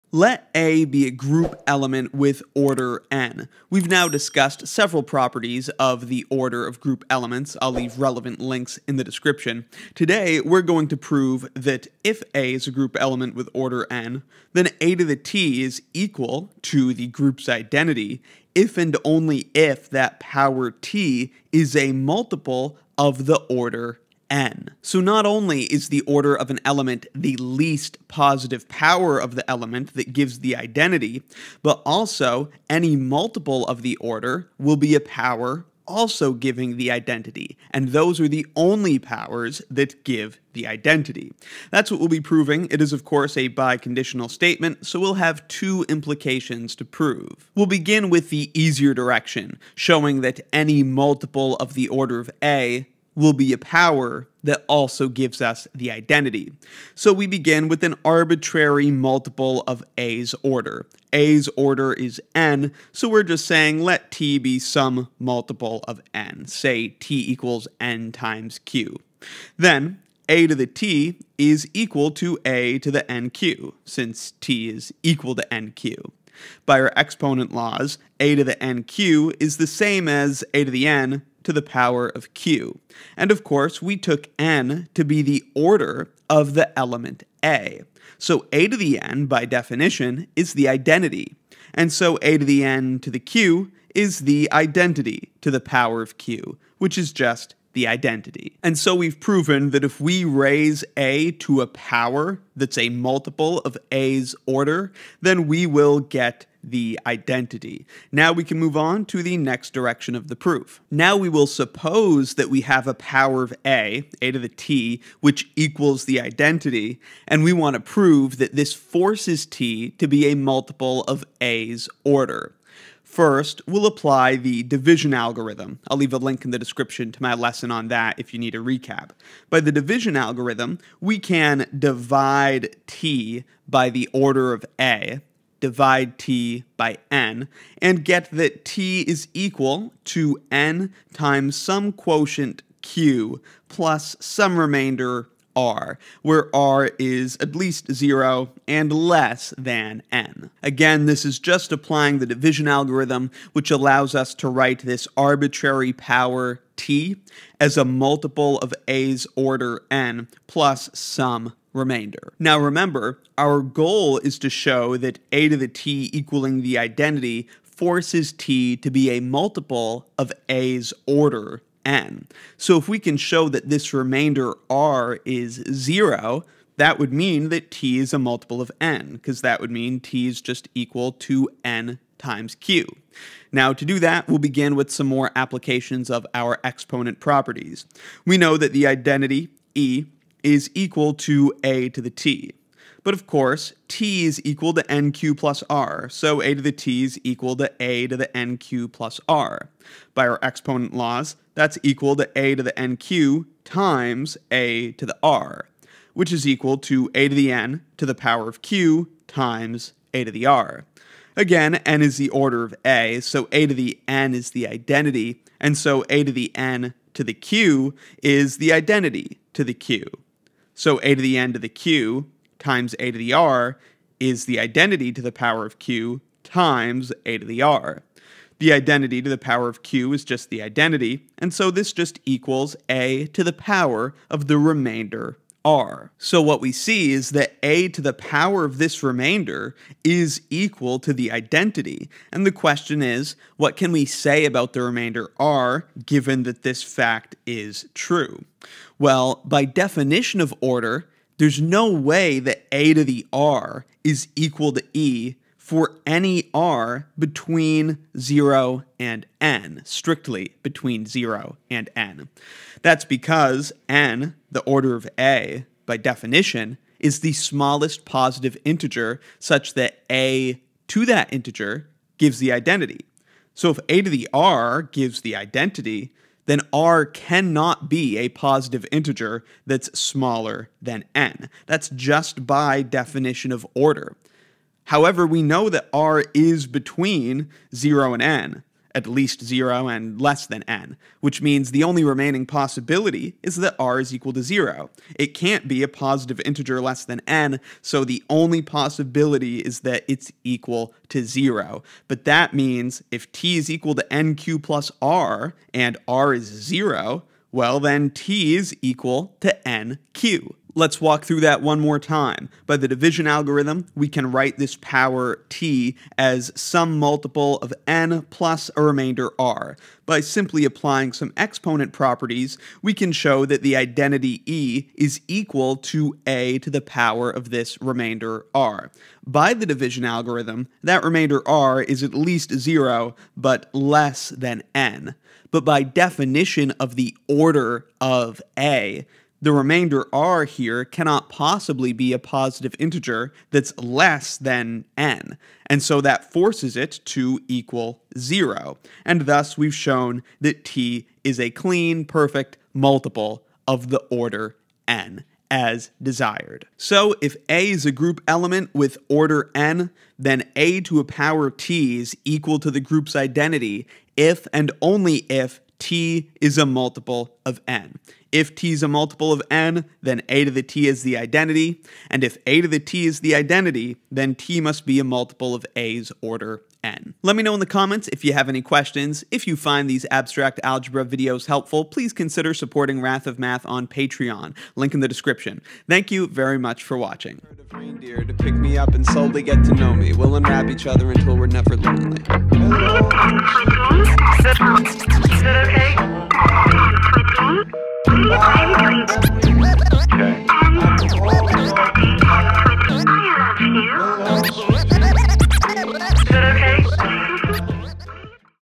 Proof: Order Multiple Powers Give the Identity | Abstract Algebra
Support the production of this course by joining Wrath of Math to access all my Abstract Algebra videos plus lecture notes at the premium tier!

If x is a group element of order n, then x^t = e if and only if t is a multiply of the order n. We prove this theorem concerning the multiples of orders directly in two steps, using the division algorithm for one direction.